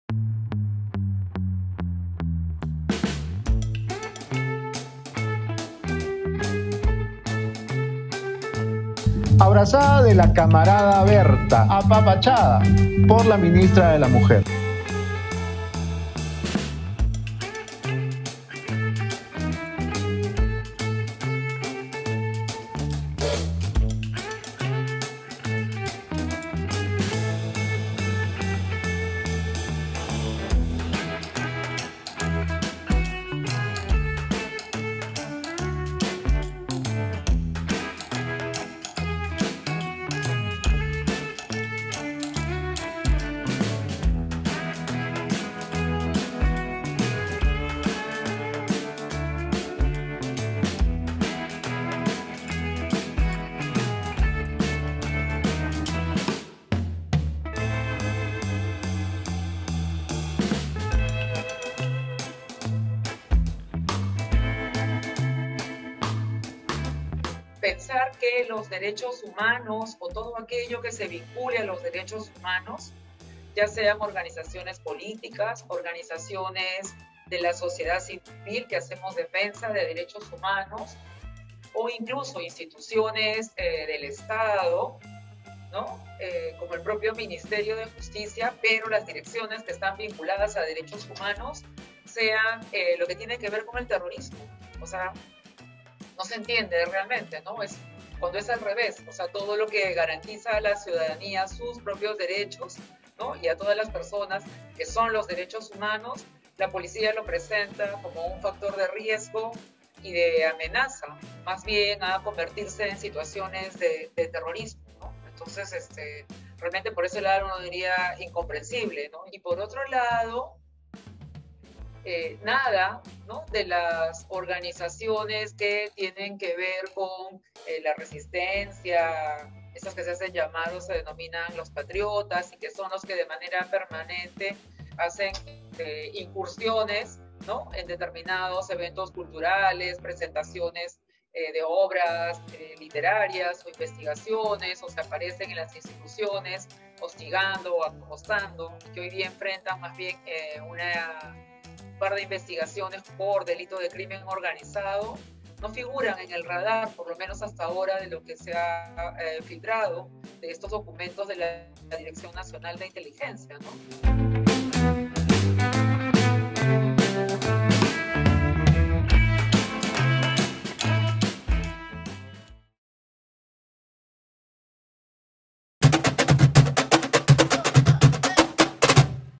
Ciberataque a la Dirección de Inteligencia del Mininter revela cuestionables investigaciones de PNP
Ataque cibernético a la Dirección General de Inteligencia del Ministerio del Interior revela cuestionables 'investigaciones’ de la Policía Nacional. Entre los documentos se considera 'amenaza terrorista' a eventos y organizaciones de derechos humanos